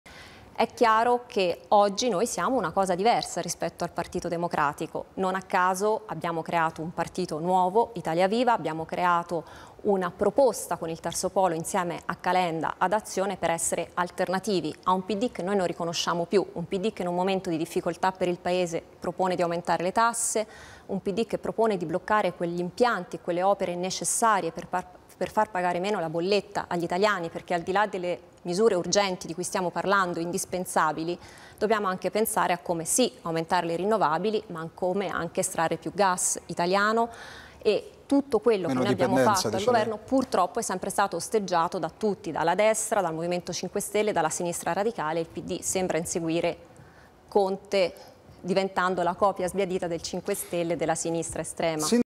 Boschi: Letta e il PD stanno diventando la copia sbiadita di Conte
ItaliaSulSerio è alternativa a questo PD che fatichiamo a riconoscere. Un PD che insegue Conte e sembra la copia sbiadita del M5S e della sinistra estrema.
Maria Elena Boschi